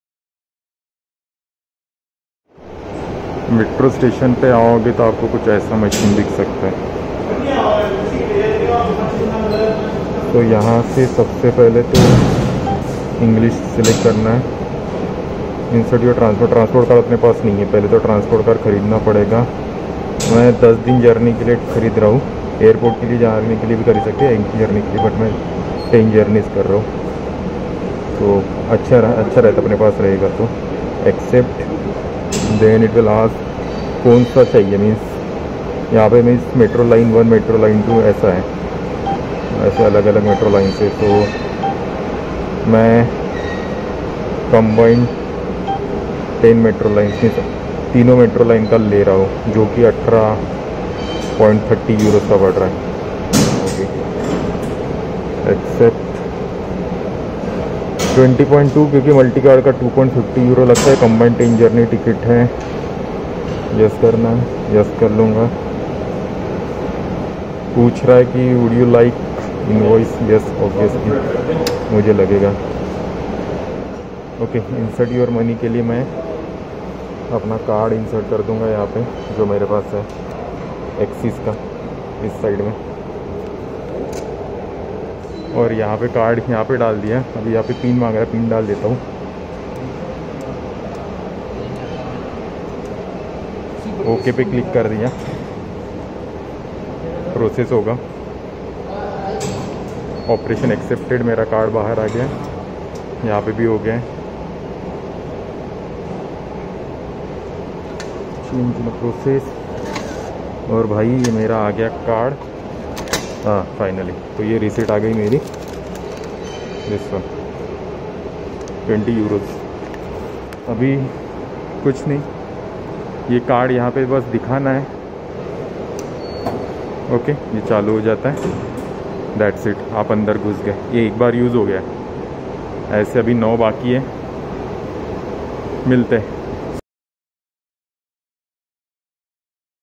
How to buy Metro-Bus-Tram tickets in Madrid, Spain?
Here's a video for you to buy Metro-Bus-Tram tickets in Madrid, Spain by using automated machine.